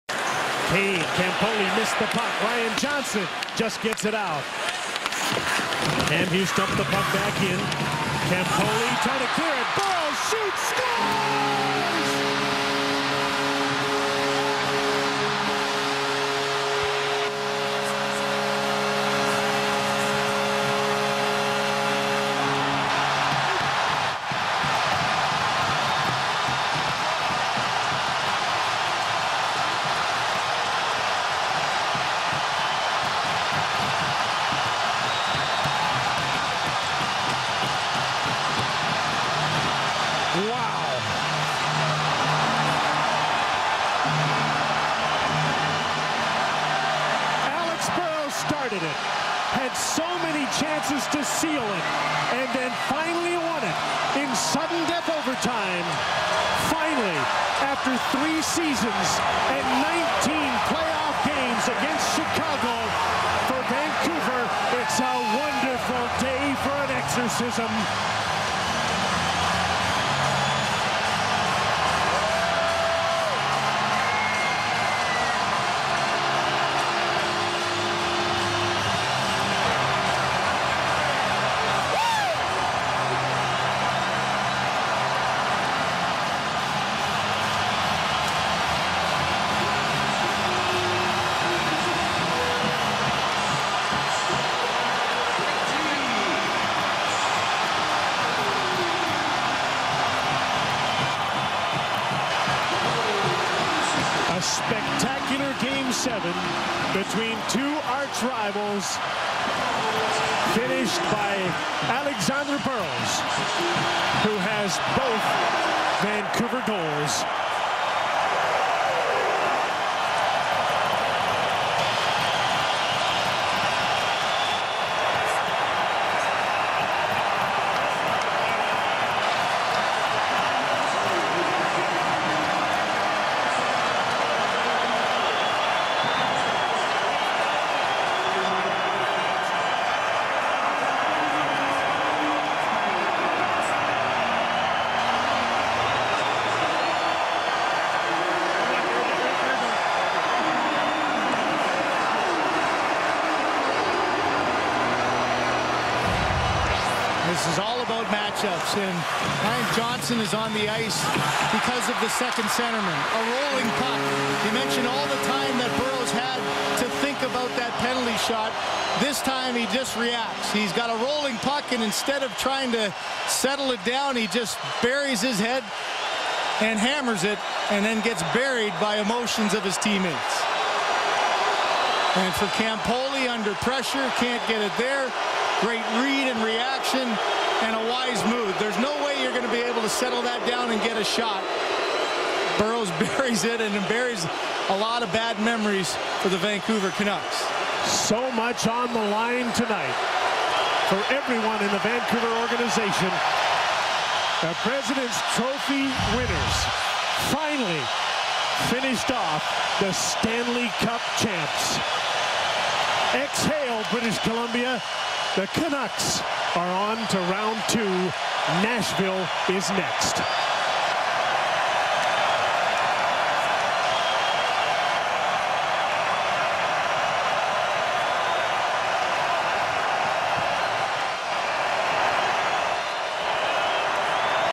Burrows OT Winner Handshakes - Canucks Vs Hawks - R1G7 2011 Playoffs - 04.26.11
Vancouver Canucks, 2010 Stanley Cup Round 1 Game 7 Winners!